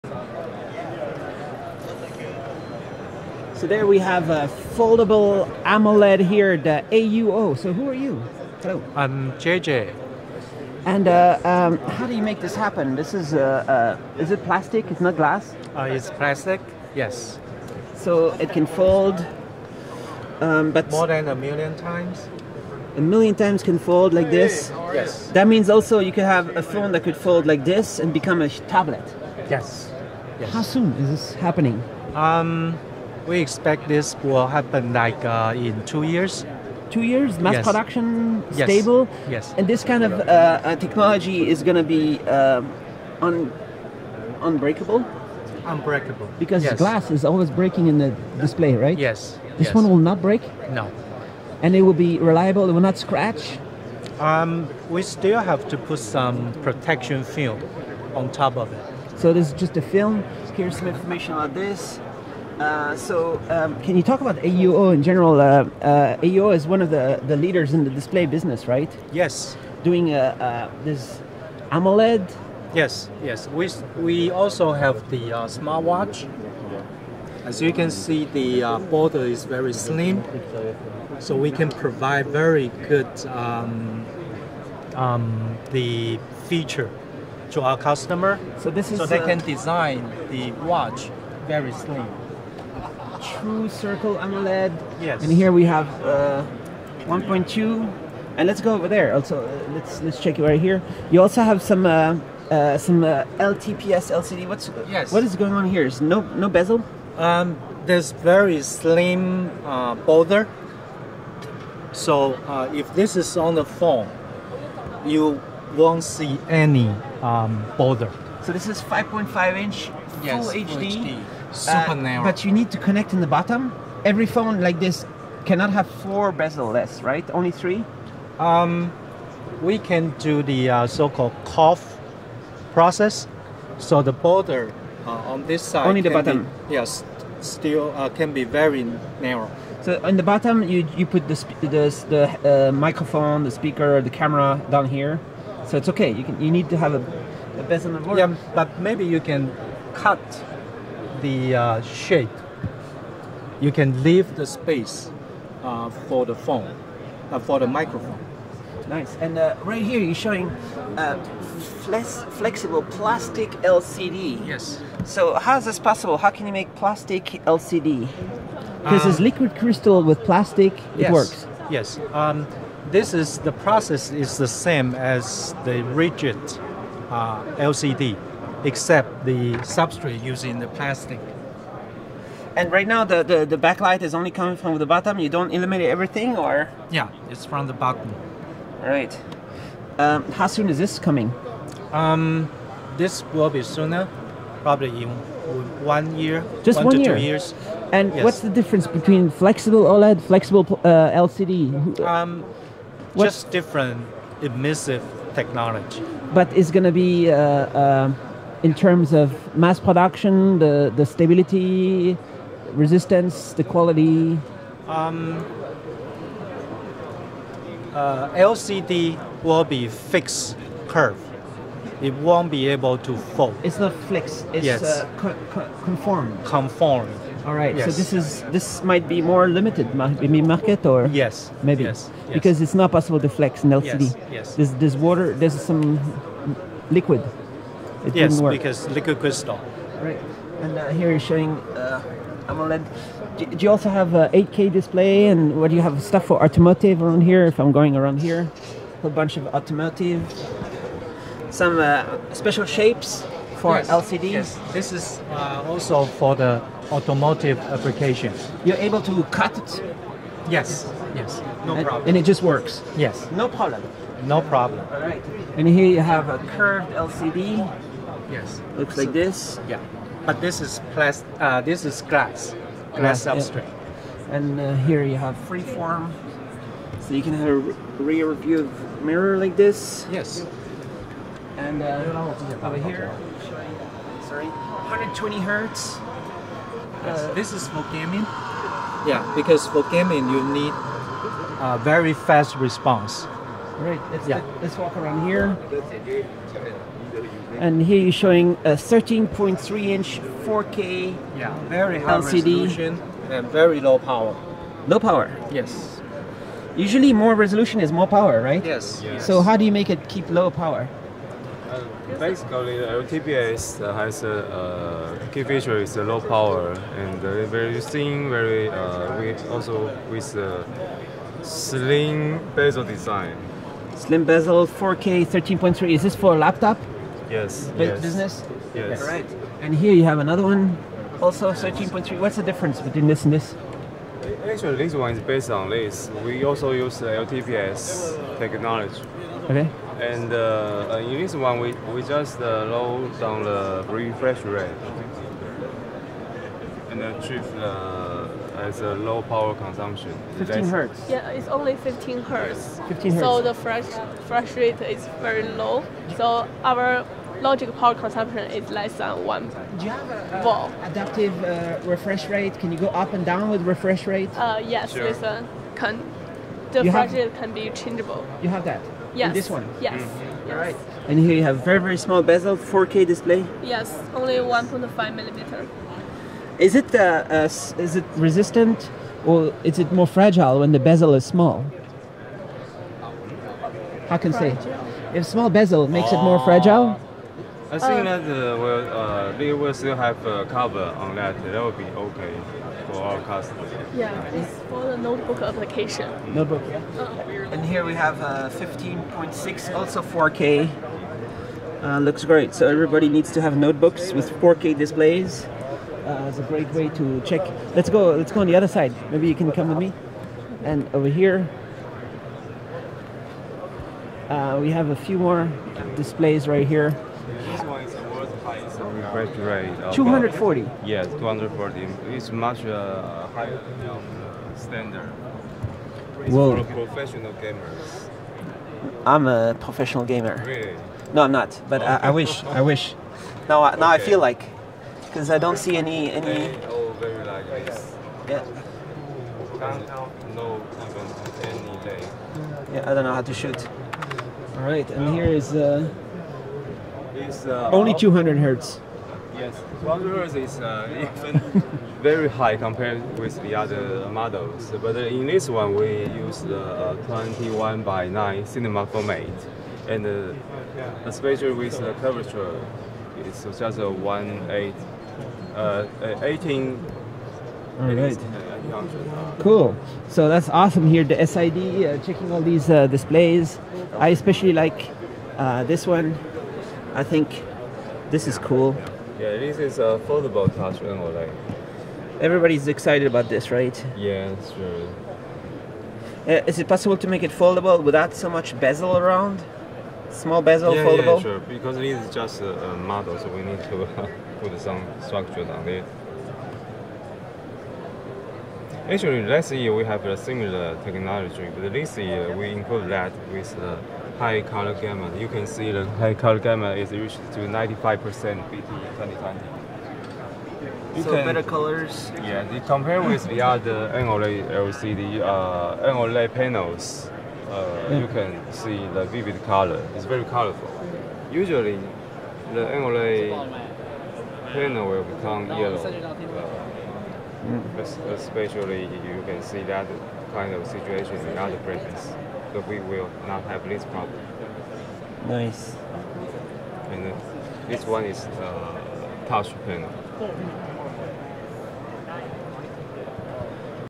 AUO shows Foldable AMOLED, Conformable Plastic LCD, Bezel-less displays and 240Hz Monitor
AU Optronics R&D Manager JJ Lih presents some of AUO's latest displays such as their 5" 1280x720 Foldable Touch AMOLED, 1.4" and 1.2" High Resolution True Circular small bezel AMOLED 454x454 for the Smartwatch industry, 5.5" FHD Super Narrow Border 0.4mm LTPS LCD, 3.5" 480x320 Plastic Flexible Conformable LCD to be available on the market within one year. 12.3" FHD AMOLED for in-car Cluster, 12.3" Free-form LTPS LCD for in-car Cluster. 9" 1280x800 Curved LCD. 8.9" Free-form LCD for Rear-view Mirror. 15.6" QHD 120Hz LTPS LCD for Gaming. 13.3" 4K Ultra-low Power LTPS LCD using lower 15hz frequency. 13.3" 4K Ultra Narrow Border 1.5mm LTPS LCD. 15.6" 4K Ultra Narrow Border 2mm LTPS LCD. AUO also shows their 240Hz World's Highest Refresh Rate Gaming PC Monitor.